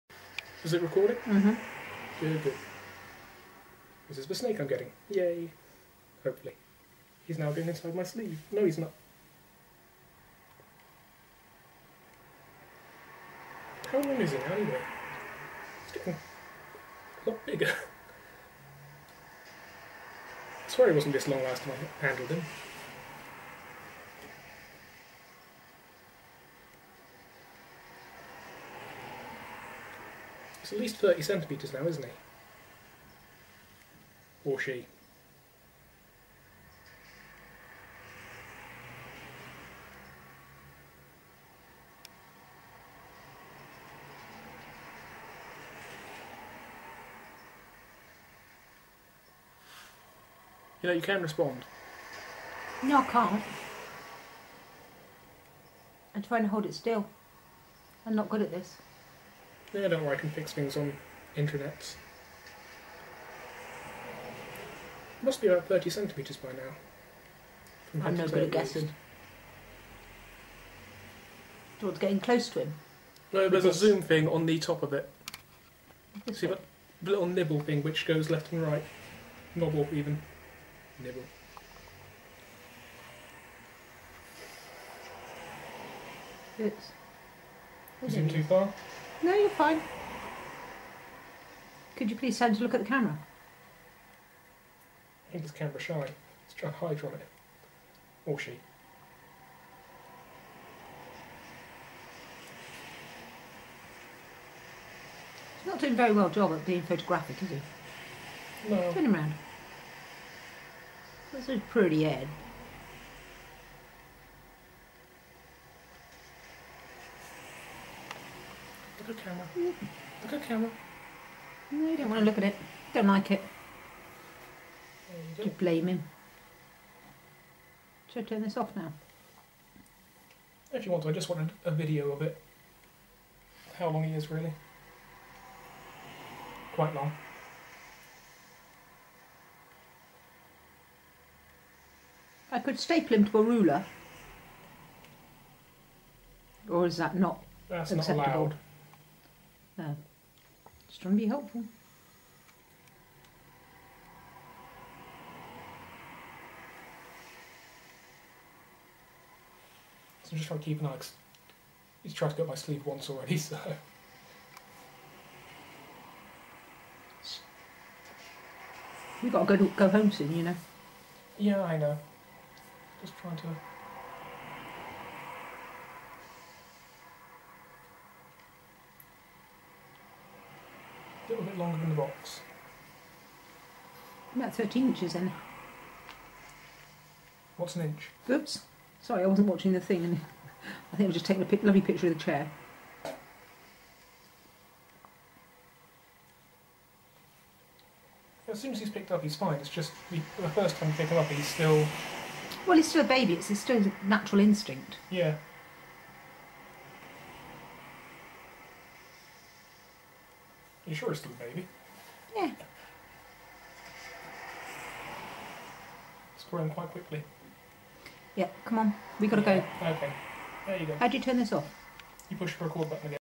First snake
HD version of the first snake video I uploaded a few weeks back.

I'm pretty new to the whole snake thing (wanted one for over a decade though) so correct me if any of this info sounds wrong

This is an anerythristic corn hatchling. It was born from a double clutch and hatched around my birthday (October 23rd) not sure exactly when, but we're just saying it was the same day

Anyway he had his first shed ages ago, and he was effectively mine in mid november. He's still being kept at the store for now, and i'm going round each day to see / handle him (or her) came from a clutch of 8 eggs.

The parent snakes (no idea if there's a propper term for them) were a snow corn, and a carolina which they believe may have had a bit of Amel in it.

looks to be about a foot long now, still no clue if its a male or female, right now its called Bob until we find out the gender. That's about all I can say for now

They're still a bit nippy when I go to pick them up, but nowhere near as nervous as they used to be. Probably doesn't help that i'm a little nervous too, but soon as they're on my hand they're perfectly happy.